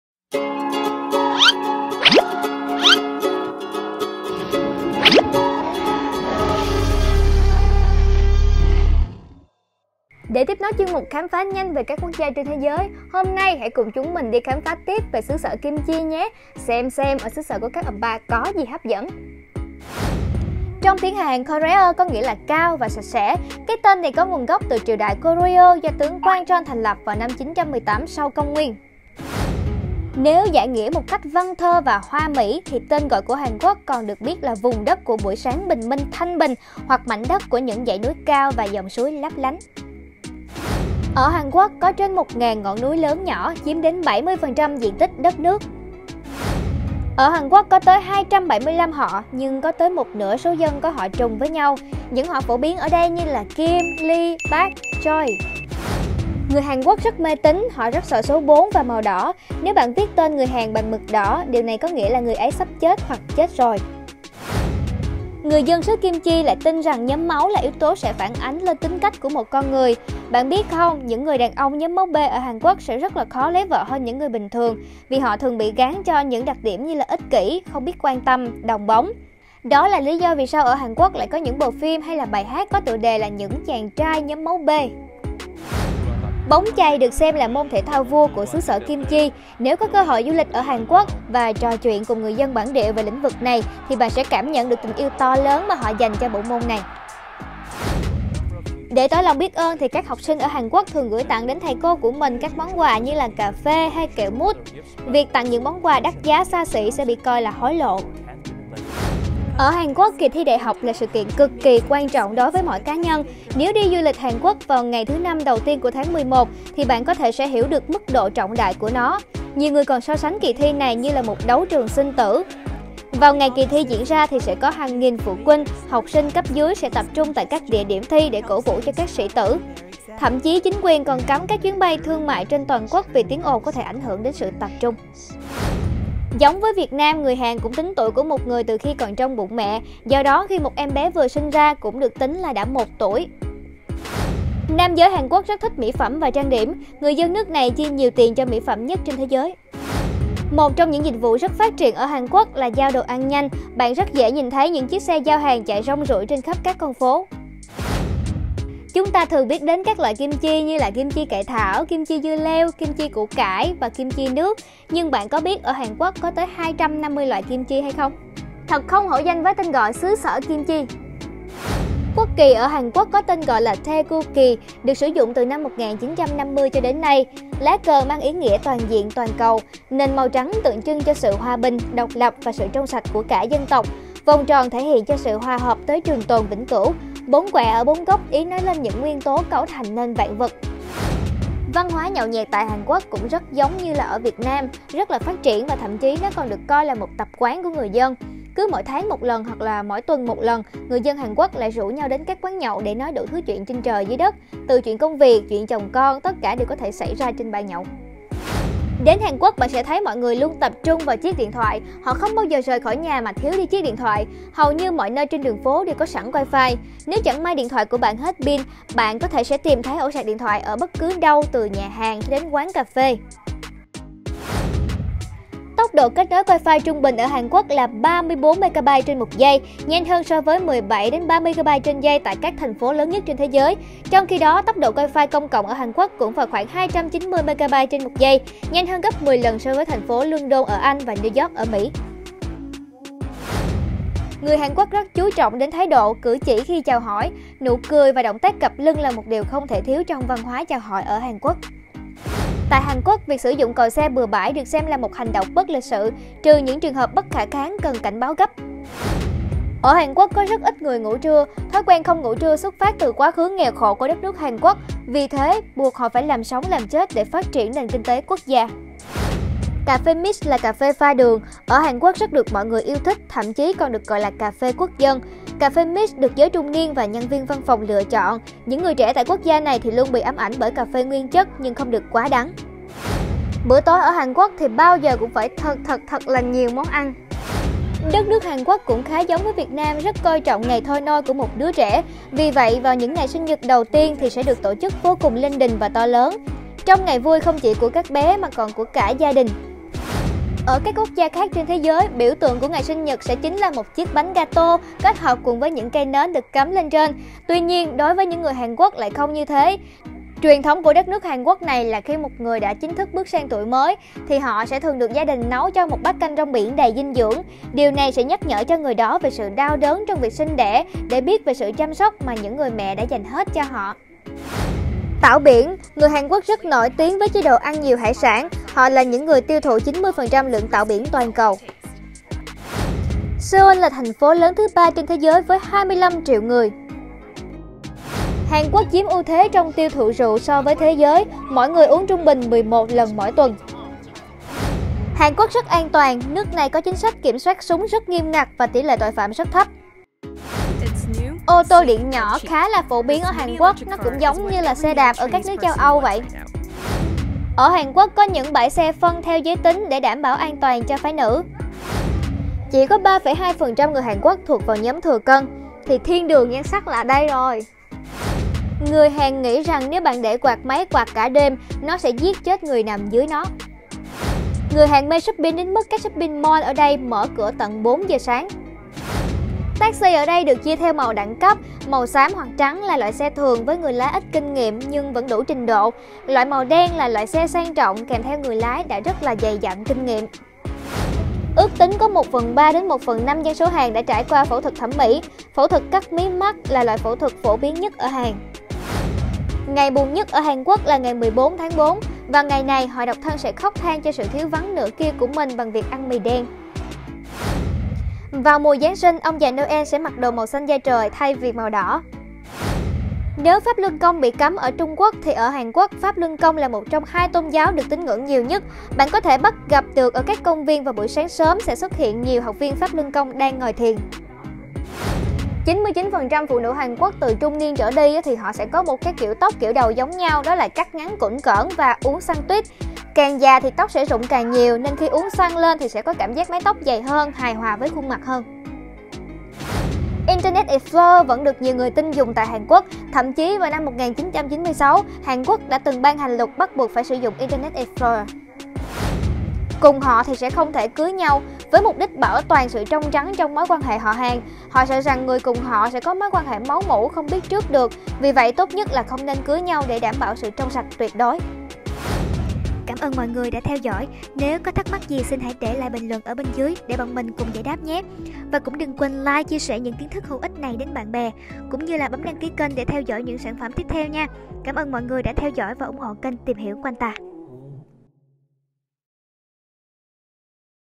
Hàn Quốc - Tin Vào Nhóm Máu Sẽ Thể Hiện Được Bản Chất Con Người | Tìm Hiểu Quanh Ta
Hôm nay chúng ta đi tìm hiểu về đất nước Hàn Quốc, xứ sở của các op pa nha, không chỉ là có cảnh đẹp thôi đâu, mà rất nhiều phong tục của người Hàn cũng tương đồng với Việt Nam đó.
0:00 - Giới thiệu
0:20 - Nội dung chính
Những kiến thức này bọn mình thu thập từ các nguồn trên mạng, nếu có gì sai sót mong các bạn bình luận bên dưới để mình biết nha. Và chúng mình rất mong nhận được đánh giá từ các bạn.
-Voice: Mai Thủy Tiên
-Dựng: Huỳnh Việt

We do NOT own all the materials as well as footages used in this video.